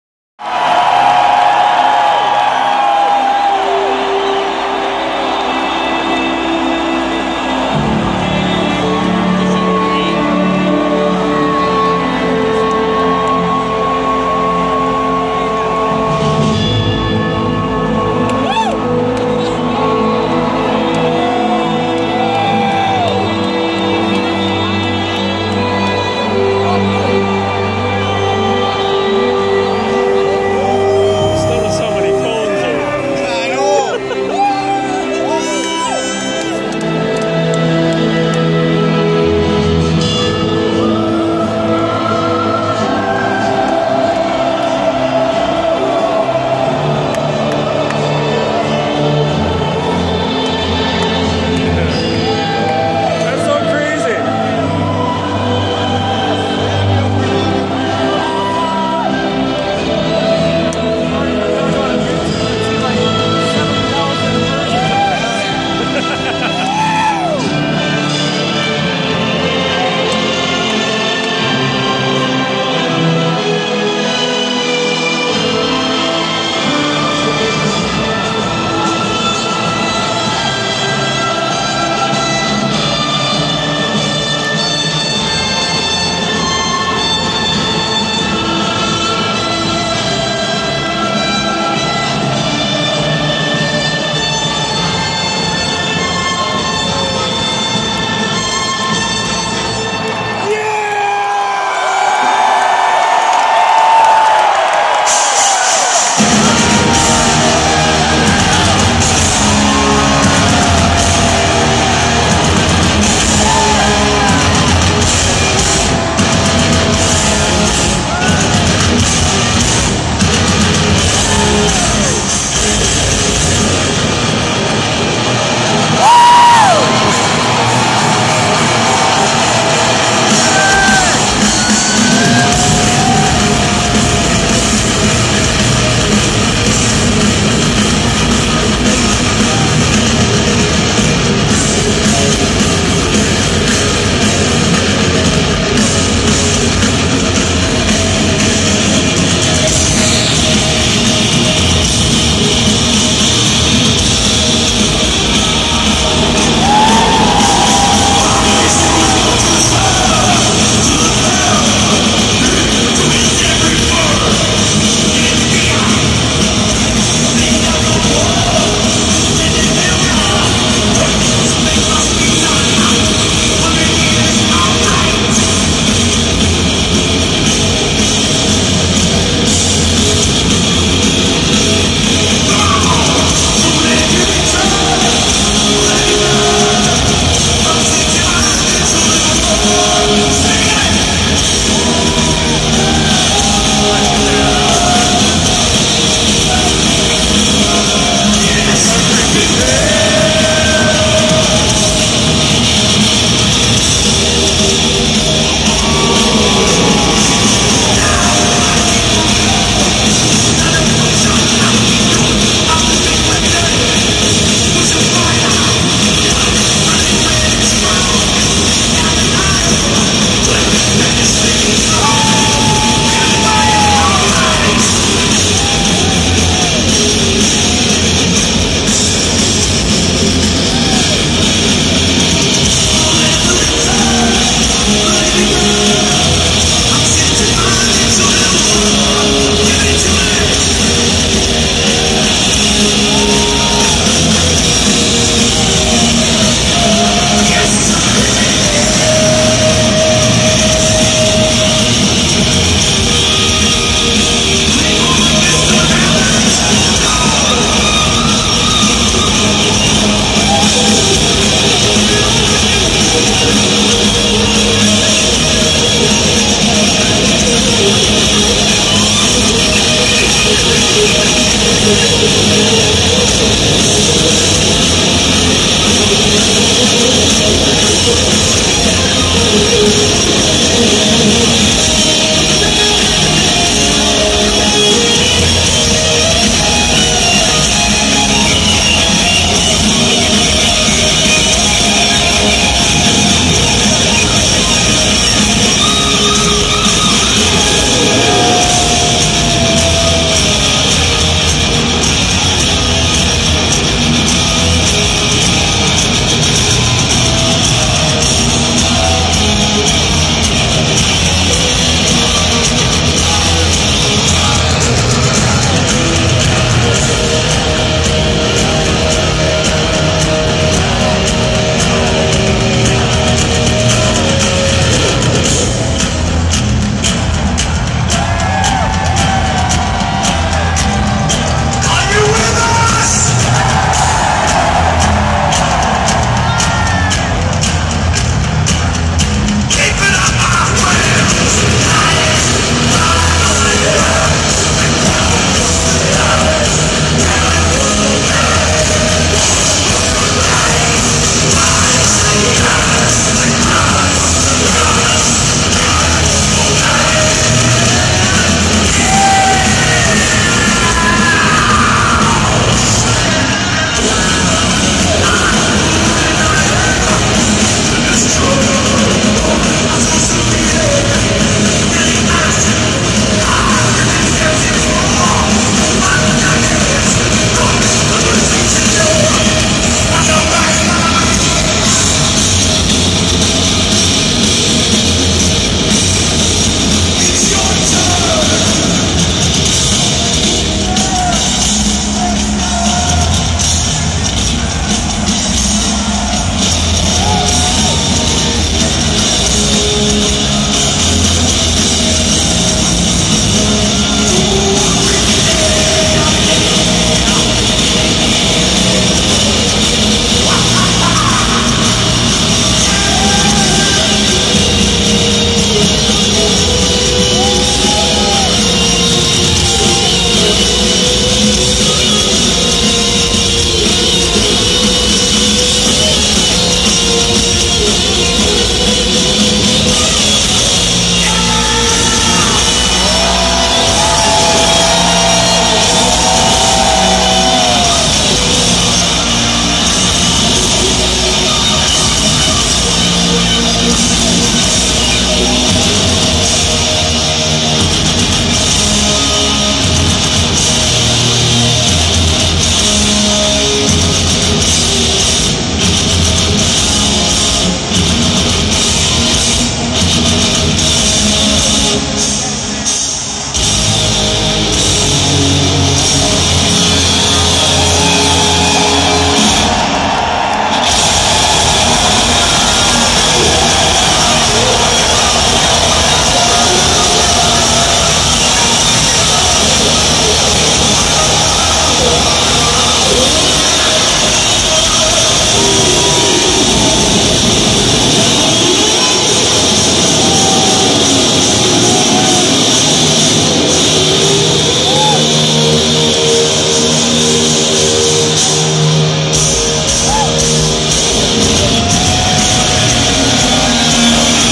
Metallica opening song - first rock concert at the new US BANK Stadium - Saturday, Aug 20th 2016
Saturday Aug 20th, 2016 - First rock concert at the US Bank Stadium in Minneapolis, MN - Metallica! Volbeat and Avenged Sevenfold opened.